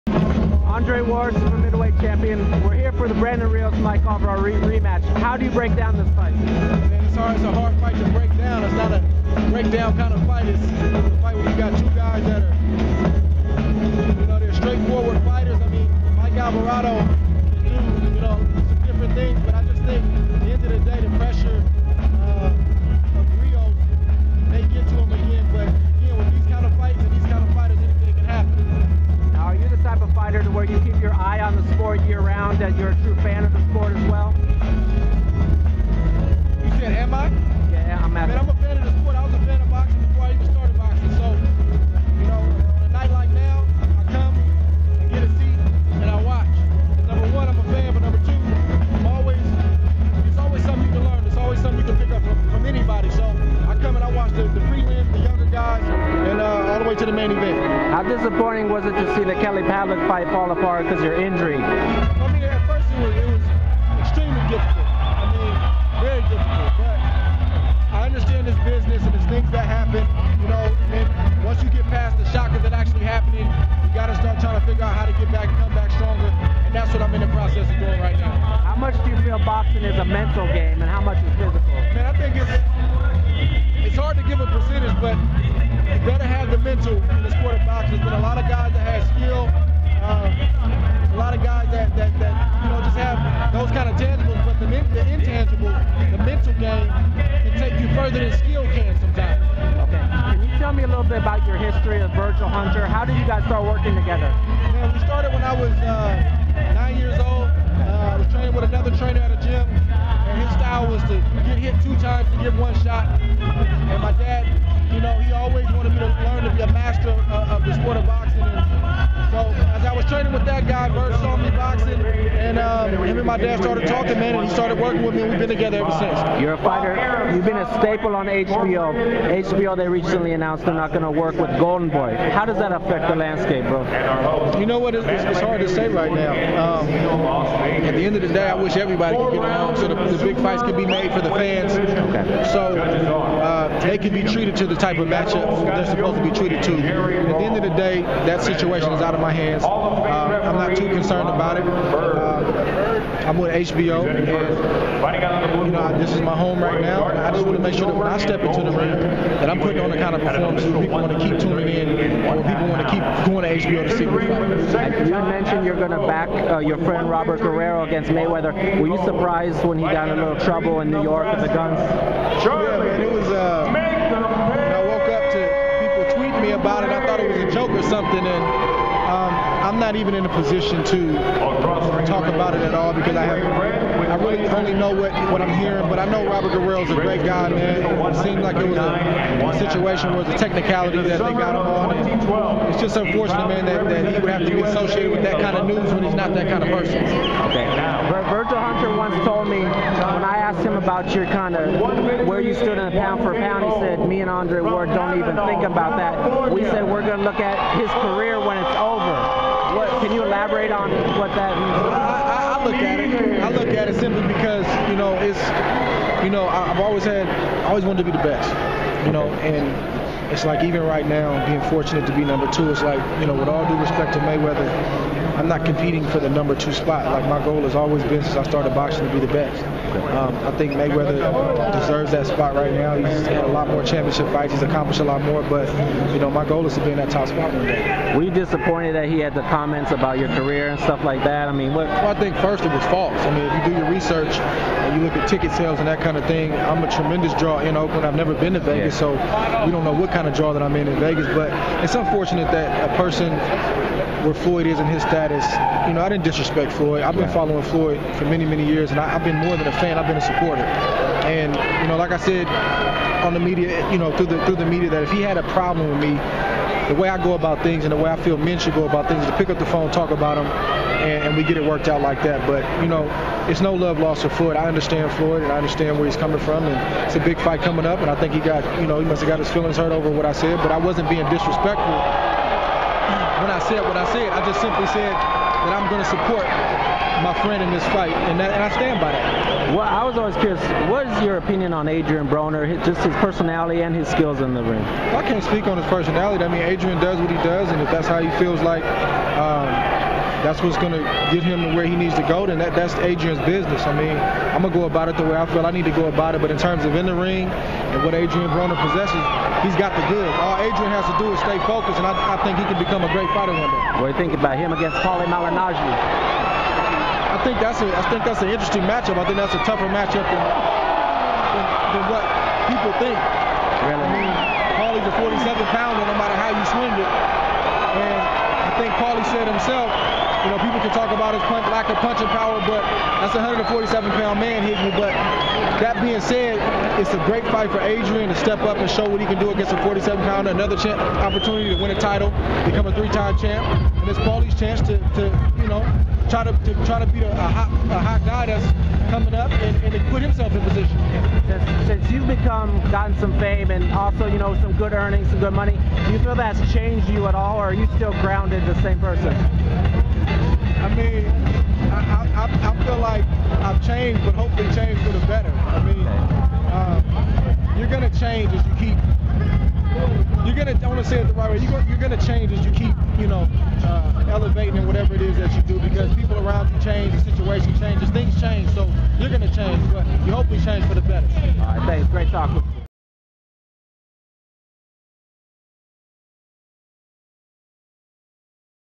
Andre Ward talks Rios-Alvarado II, Floyd Mayweather's comments, the HBO-Golden Boy drama, more
Chris Robinson catches up with super middleweight world champion Andre Ward inside of the Mandalay Bay in Las Vegas, Nevada, as he discusses the Brandon Rios-Mike Alvarado rematch, the recent comments that Floyd Mayweather made questioning his popularity, the HBO-Golden Boy drama, his bond with trainer Virgil Hunter, his career learning lessons, and more